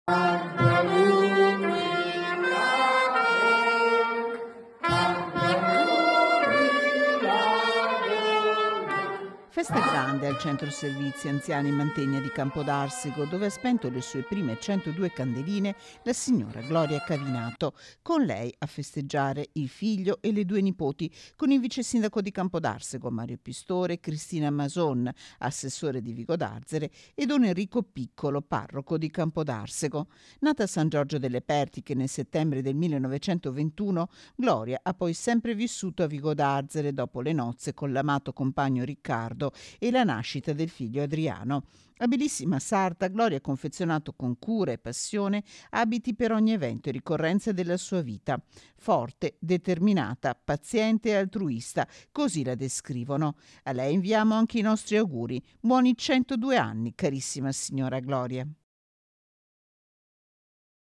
LA SIGNORA GLORIA SPEGNE 102 CANDELINE | 23/09/2023
TG Bassano del Grappa 23/09/2023 - CAMPODARSEGO - Davvero un bel traguardo quello a cui è arrivata la signora Gloria Cavinato festeggiata nel giorno del suo 102° compleanno.   || Festa grande nel centro servizi anziani Mantegna di Campodarsego dove ha spento le sue prime 102 candeline la signora Gloria Cavinato. Con lei a festeggiare il figlio, e le due nipoti, con il vicesindaco di Campodarsego Mario Pistore, Cristina Mason, assessore di Vigodarzere e don Enrico Piccolo, parroco di Campodarsego. Nata a San Giorgio delle Pertiche nel settembre del 1921, Gloria ha poi sempre vissuto a Vigodarzere, dopo le nozze con l'amato compagno Riccardo e la nascita del figlio Adriano. Abilissima sarta, Gloria ha confezionato con cura e passione abiti per ogni evento e ricorrenza nella sua vita. "Forte, determinata, paziente e altruista" così la descrivono. A lei inviamo anche i nostri auguri: "Buoni 102 anni, carissima signora Gloria". (Servizio di Chiara Gaiani) - Segui Rete Veneta anche sul digitale terrestre! Visita il sito